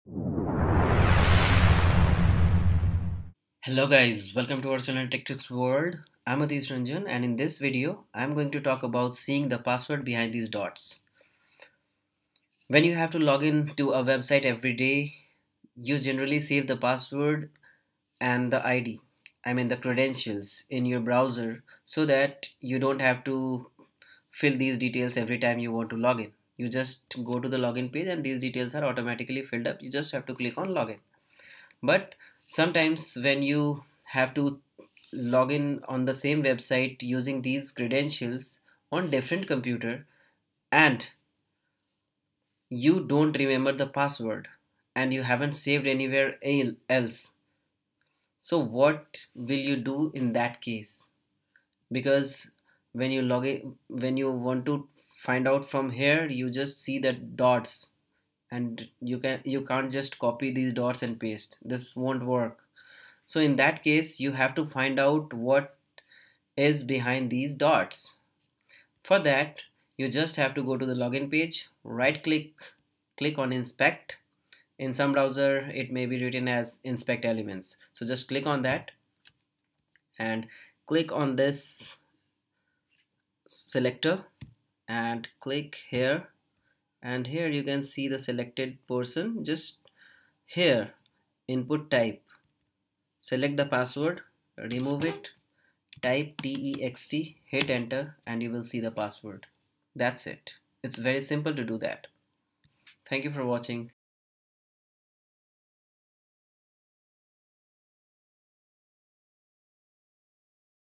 How to See the Password Hidden Behind the Dots (Asterisks)?
When we save the password on few website's login pages so that we do not have to enter the credentials again and again, then we most likely forget the password because we do not type it all the time. And, if that is not saved somewhere, then it can be problematic when you have to use that password on some other computer. This video explains how you can uncover the dots to see the password behind that.